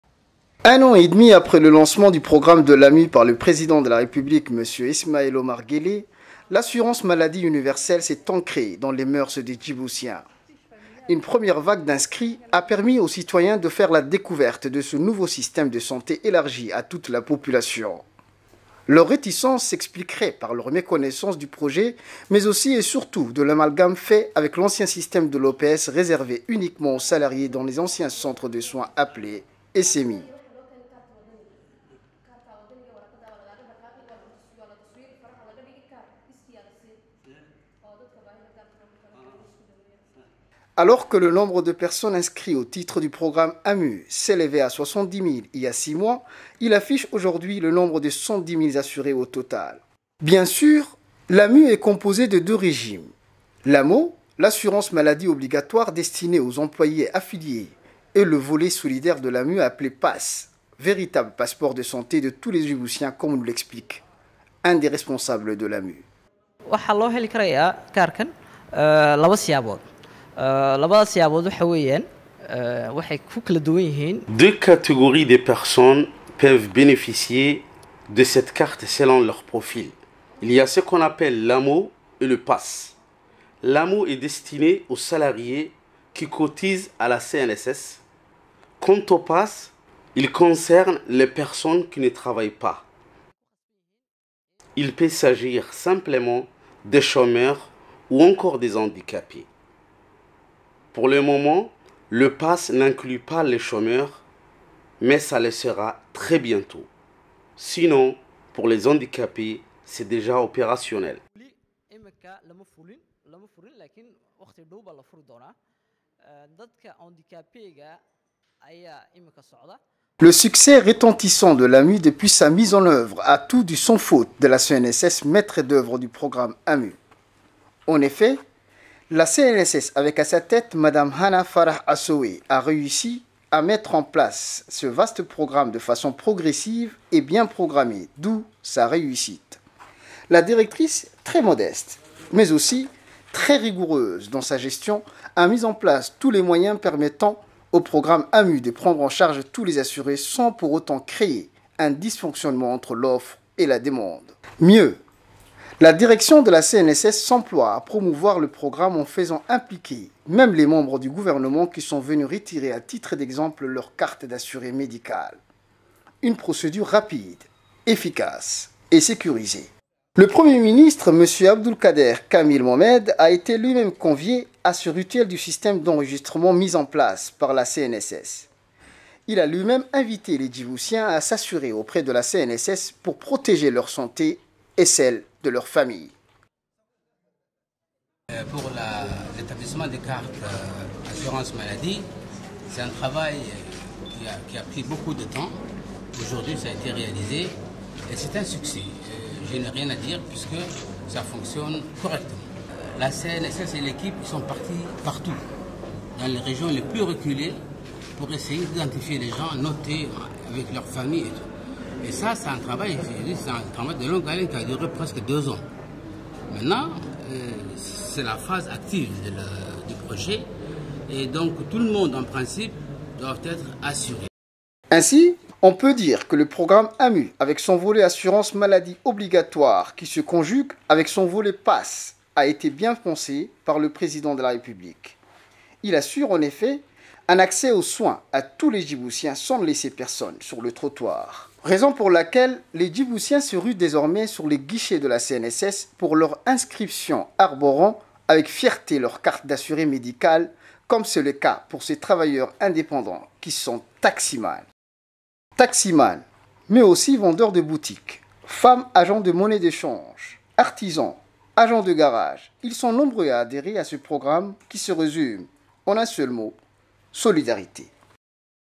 enregistrement des assurés par la CNSS
Le Programme d’Assistance Santé (PASS) est le volet solidaire de l’Assurance Maladie Universelle. Les bénéficiaires sont les personnes sans ressources ou qui n'ont pas les moyens de se soigner.
Les enregistrements pour cette catégorie démunie se déroulent dans les régions de l'intérieur. Cependant , la CNSS a exceptionnellement lancé l' immatriculation DANS LA CAPITALE aux personnes à mobilité réduites et des personnes souffrantes de troubles mentaux .